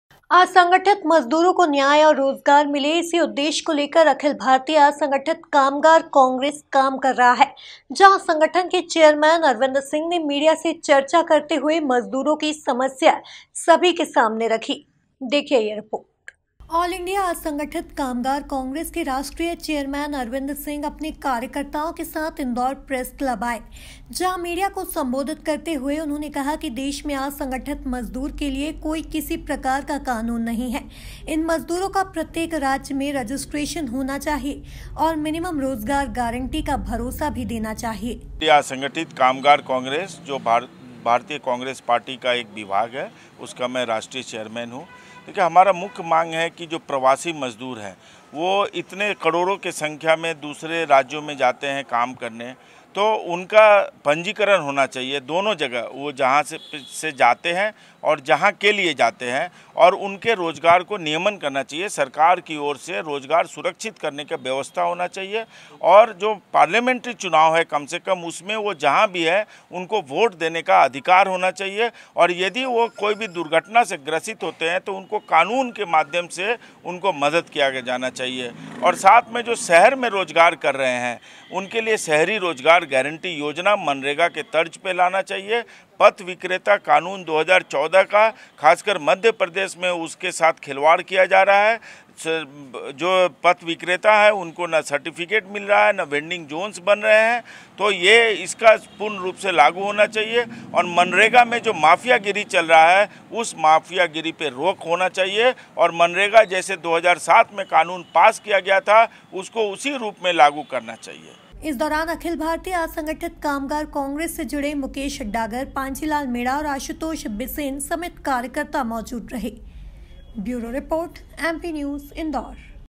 मजदूरों को न्याय दिलाने के लिए असंगठित कामगार कांग्रेस ने उठाई आवाज, सरकार से की ये मांग! MP News
मजदूरों को न्याय दिलाने के लिए असंगठित कामगार कांग्रेस ने उठाई आवाज, सरकार से की ये मांग! MP News Indore